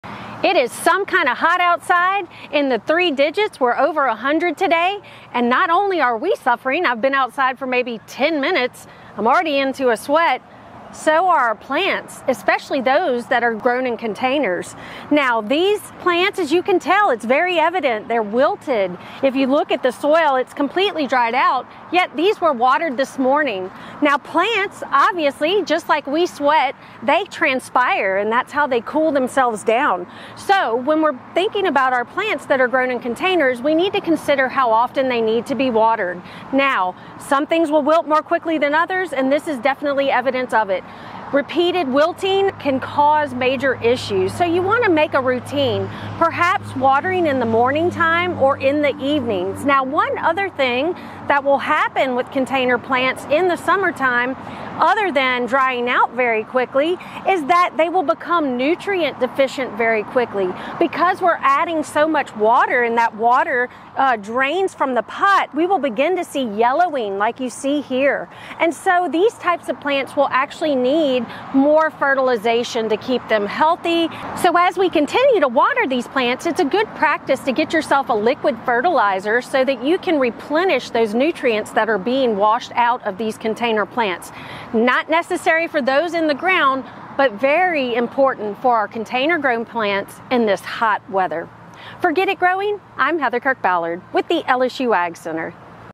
Get It Growing: How to help your container plants survive the heat
High temperatures can harm container plants — even if you’re watering them. On this edition of Get It Growing, LSU AgCenter horticulturist Heather Kirk-Ballard explains what you can do to help your container plants survive.